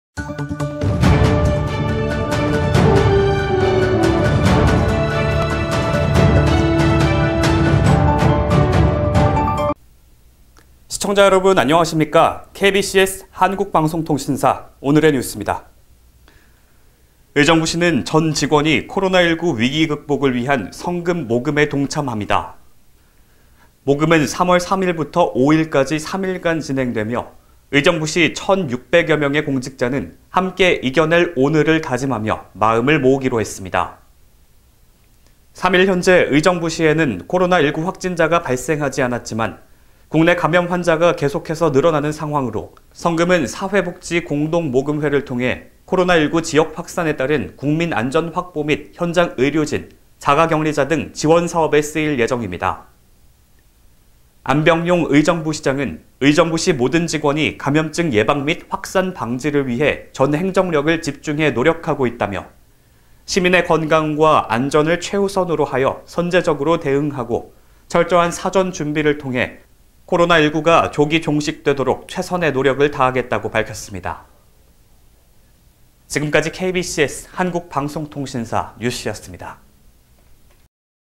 의정부시, 공무원 위기 극복을 위한 성금 모금 전개
의정부시(시장 안병용) 전 직원이 코로나19 위기 극복을 위한 성금 모금에 동참한다.

모금은 3월 3일부터 5일까지 3일간 진행되며, 의정부시 1천 6 백여 명의 공직자는 ‘함께 이겨낼 오늘’을 다짐하며 마음을 모으기로 했다.

3일 현재, 의정부시에는 코로나 19 확진자가 발생하지 않았지만 국내 감염환자가 계속해서 늘어나는 상황으로, 성금은 사회복지공동모금회를 통해 코로나 19 지역 확산에 따른 국민안전 확보 및 현장의료진, 자가격리자 등 지원사업에 쓰일 예정이다.

안병용 의정부시장은 “의정부시 모든 직원이 감염증 예방 및 확산 방지를 위해 전 행정력을 집중해 노력하고 있다”라며“시민의 건강과 안전을 최우선으로 하여 선제적으로 대응하고, 철저한 사전 준비를 통해 코로나19가 조기 종식되도록 최선의 노력을 다하겠다”라고 밝혔다.